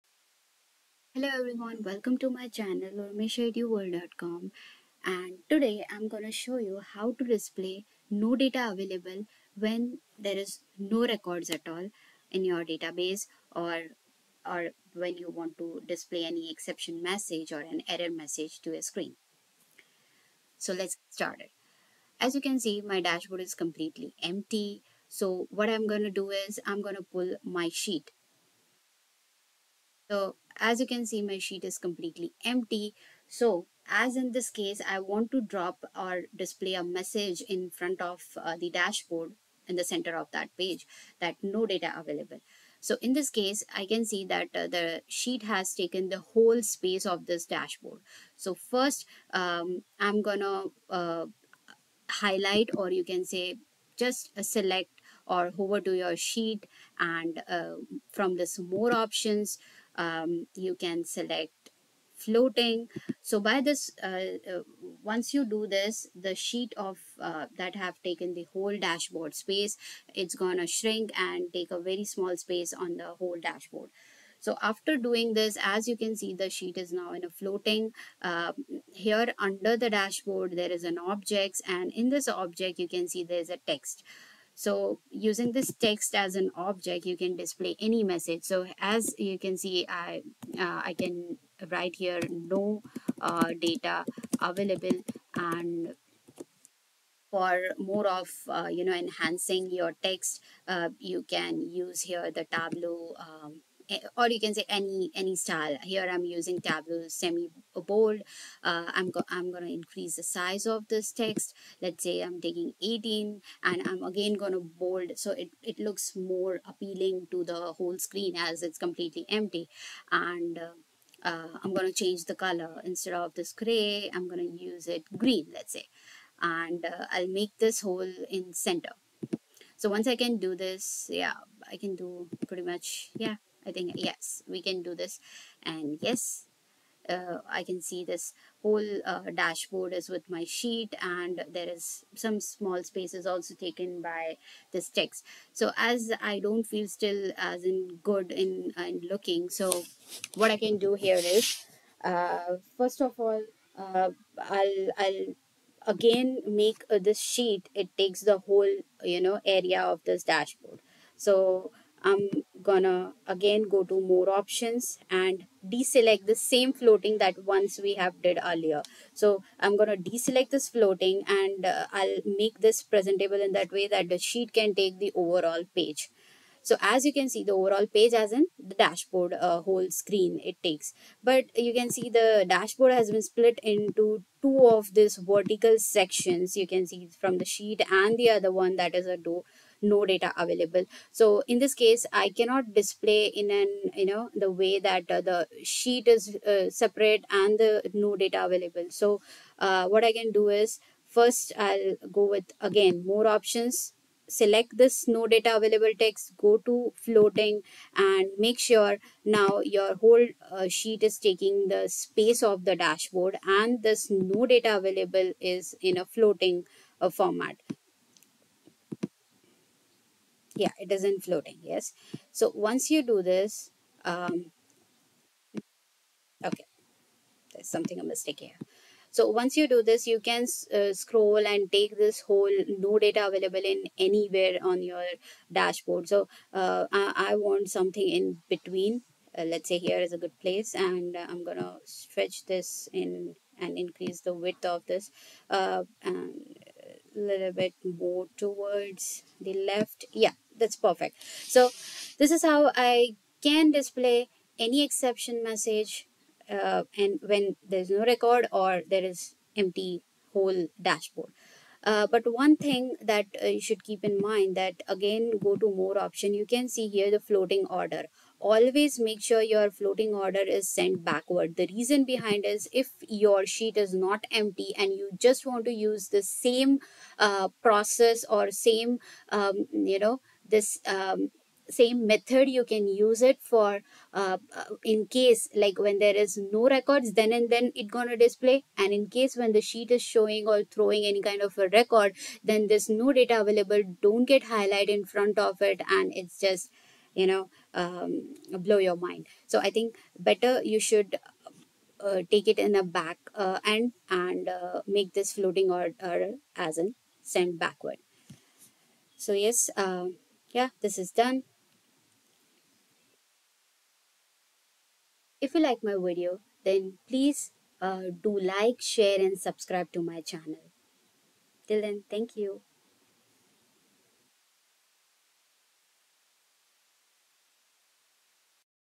How to display "No Data Available" Exception message in Tableau?tableau
This video will helps in displaying exception message in the dashboard when no records or no data available.

In business operational reports displaying such exception messages plays vital role.